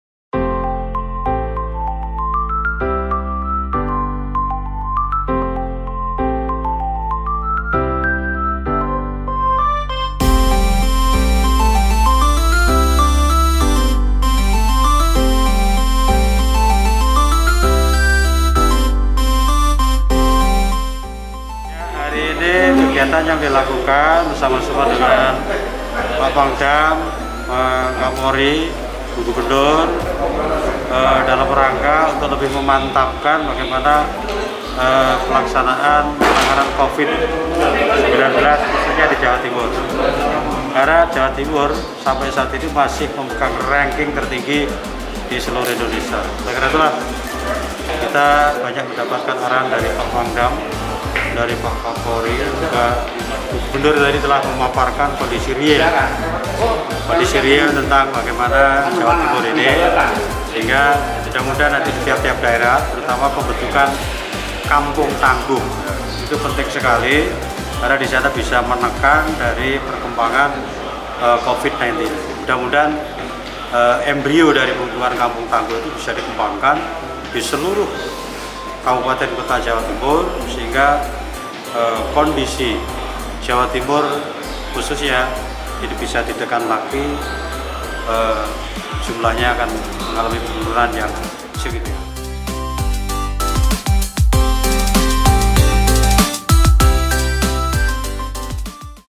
Video Conference Rapat Koordinasi Penanganan Covid-19
Video Conference Rapat Koordinasi Penanganan Covid-19 Wilayah Jawa Timur
Bersama Panglima TNI Dan Kapolri di Ruang ISC Diskominfotik Kota Blitar